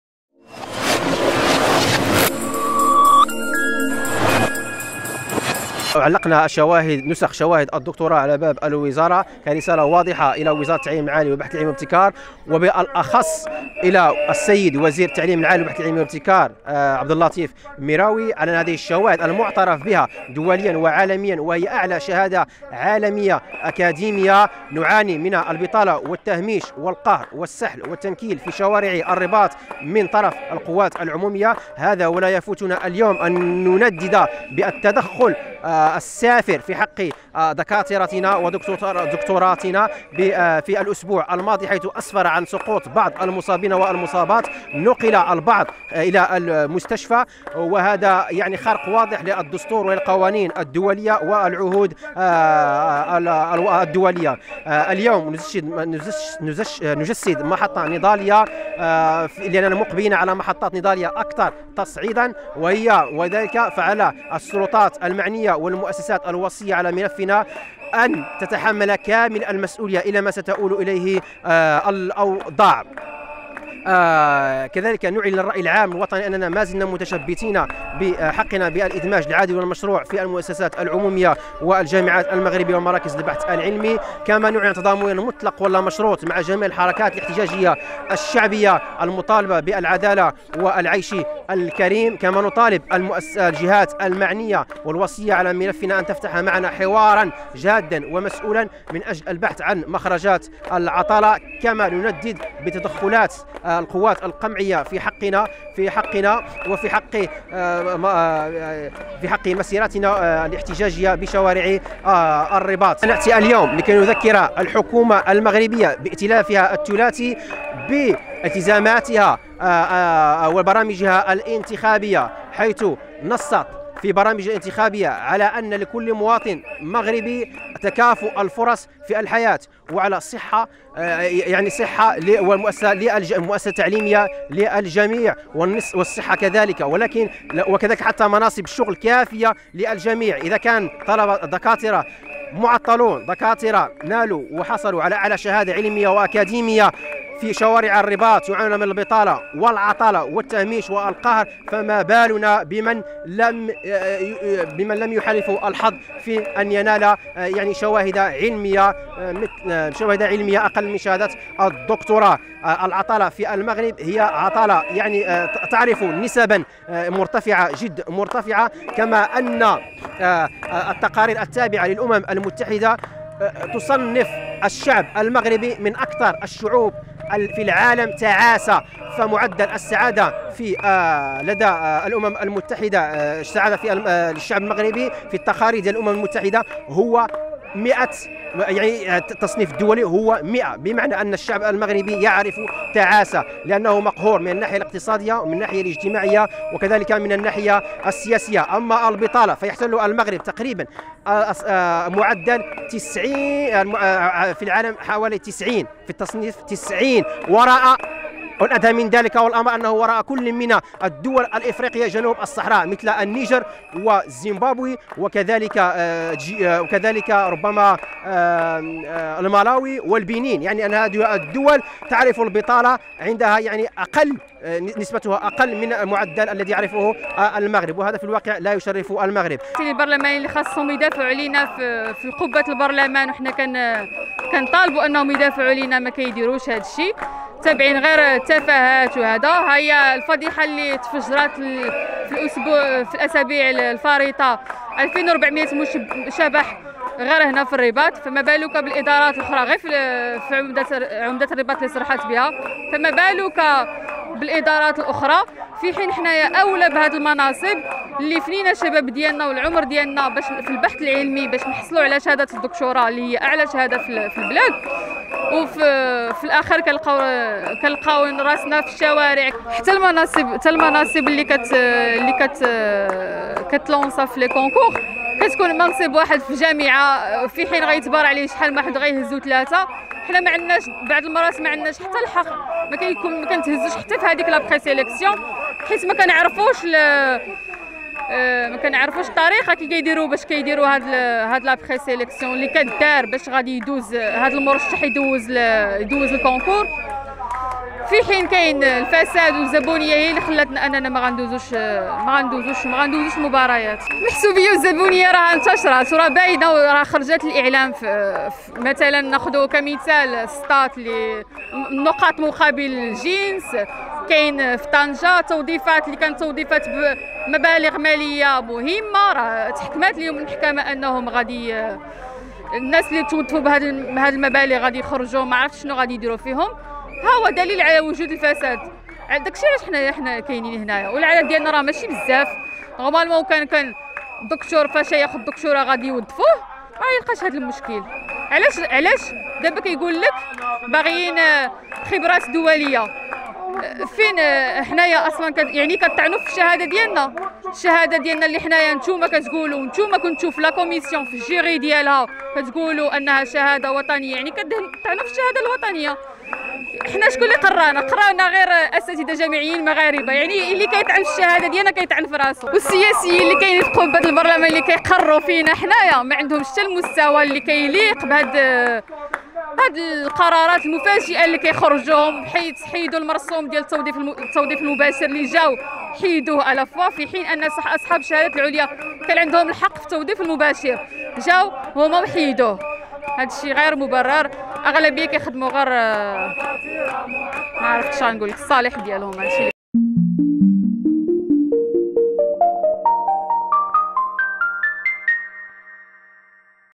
المغرب من أكثر الدول تعاسة وبطالة.. البرلمانيين اللي كيقررو فمصيرنا ماعندهمش شواهدنا
المغرب من أكثر الدول تعاسة وبطالة .. البرلمانيين لي كيقرروا فمصيرنا معندهمش شواهدنا